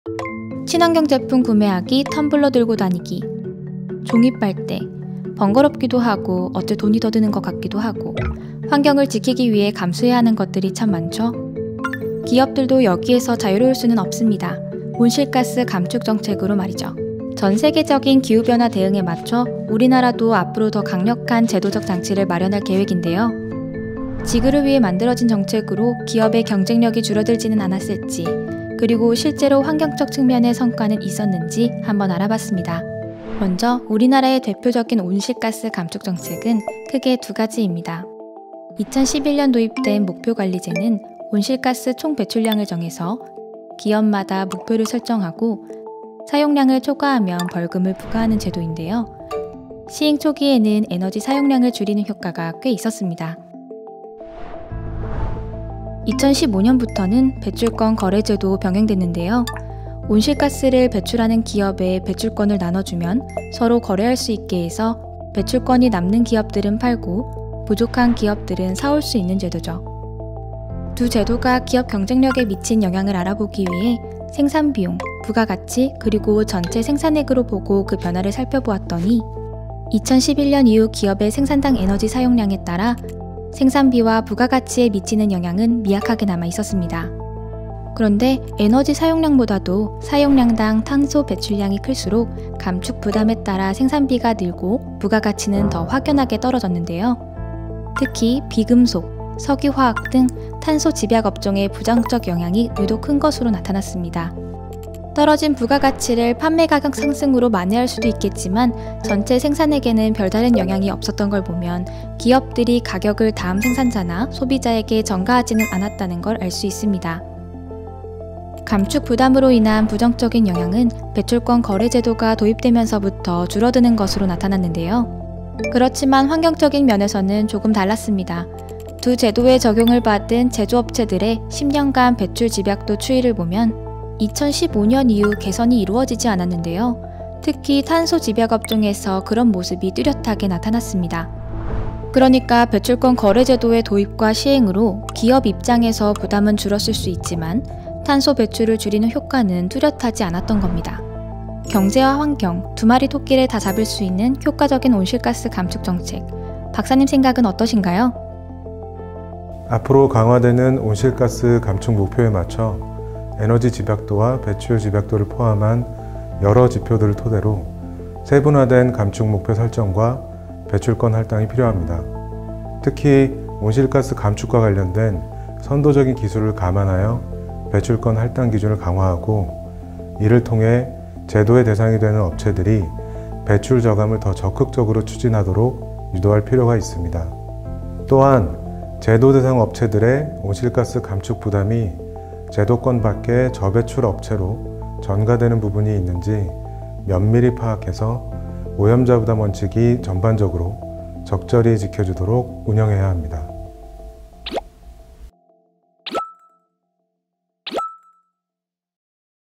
지구는 지켜야겠는데🌏... 온실가스 배출 🏭 줄이기, 기업 경쟁력에는 괜찮은걸까? (ENG SUB)
2011년부터 본격적으로 온실가스 감축정책이 기업들을 대상으로 시행됐습니다 🏭 사실 일상에서 우리가 환경을 지키려는 각종 노력들을 하는게  때론 부담스럽고 번거로울 때도 있듯이 기업들도 마찬가지일텐데요. 온실가스 감축정책으로 기업들의 경쟁력이 줄어들진 않았을지, 그리고 환경적인 성과는 어땠는지, 알아볼까요? 🕵️‍♀️

🔍 관련 보고서
온실가스 감축정책이 제조업 경쟁력에 미치는 영향

🔍 저자 : 김현석 KDI 연구위원 👉 저자 인터뷰 바로가기 2:40

🔍 관련 영상
① 세션3 토론｜탄소중립을 위한 산업·기술 분야 협력 강화｜제4차 미래전략컨퍼런스